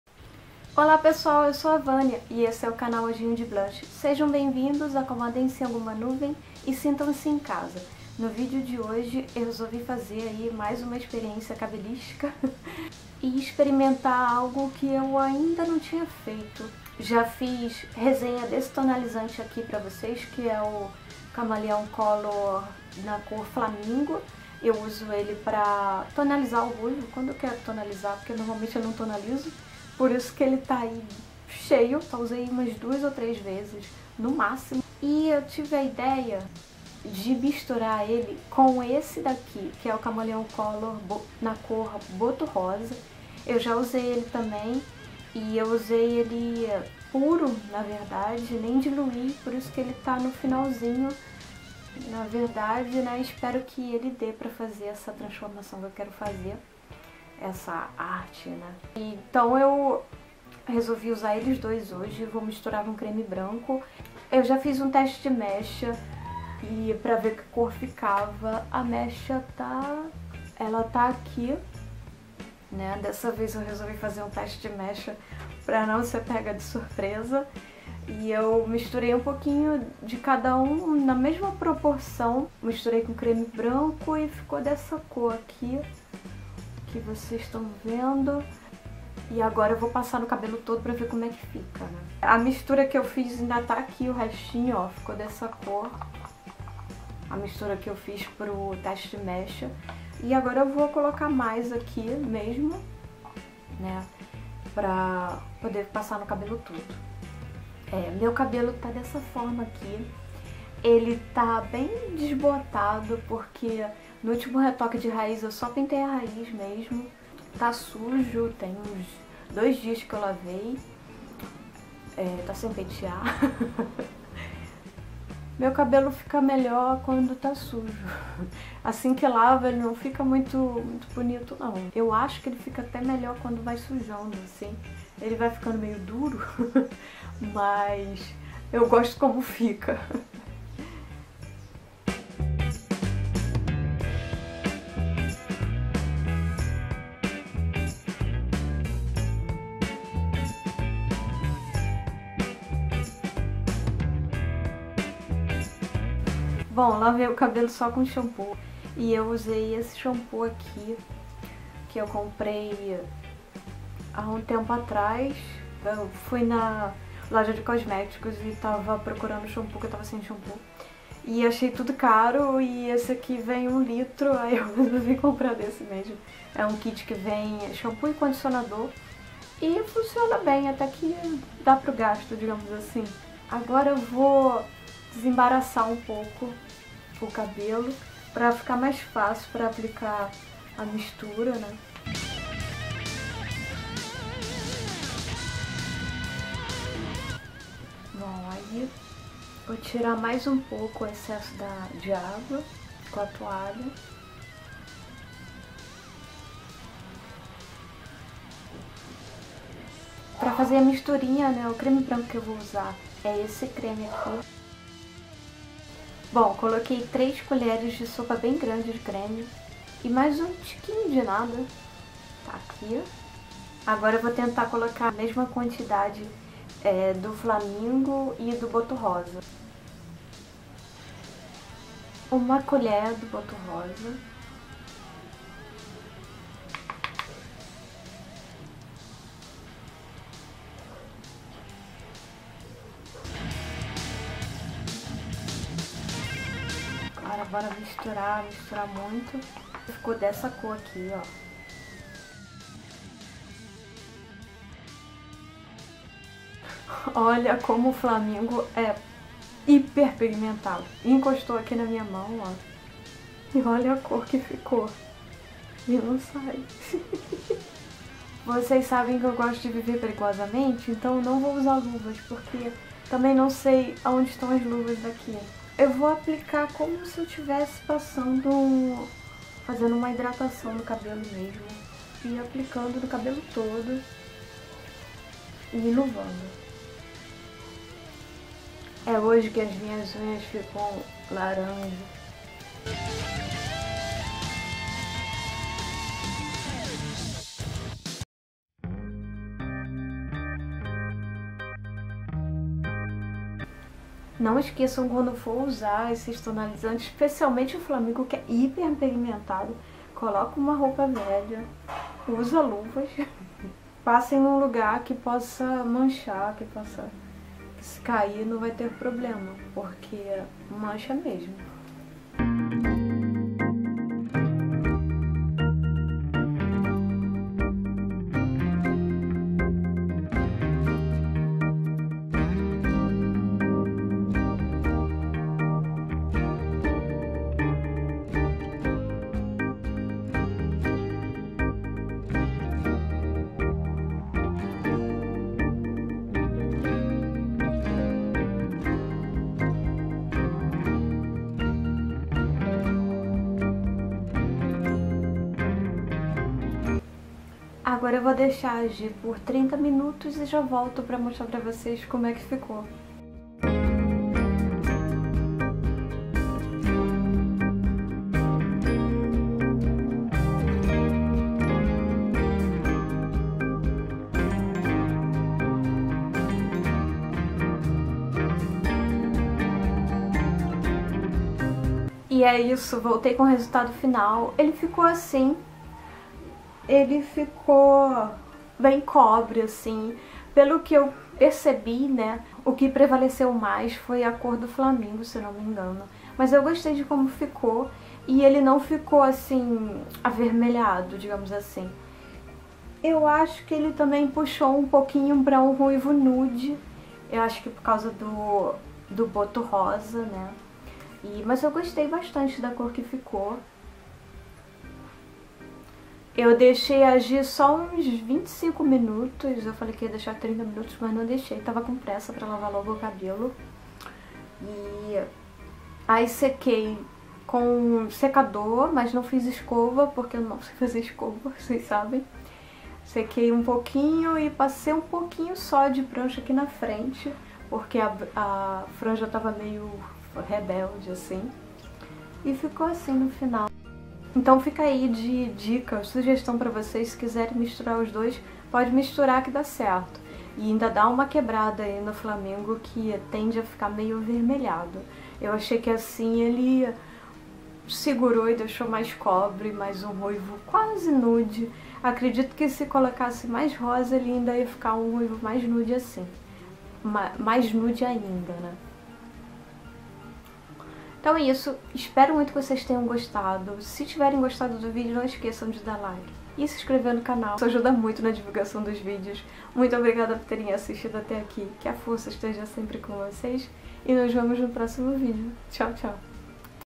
TONALIZANDO O RUIVO COM KAMALEÃO COLOR FLAMINGO E BOTO ROSA
No vídeo de hoje eu tonalizei o meu cabelo ruivo com os dois tonalizantes da Kamaleão color Flamingo e Boto Rosa junto, misturados com creme branco.

Câmera: Nikon P520

Editor: Adobe Premiere Pro CS6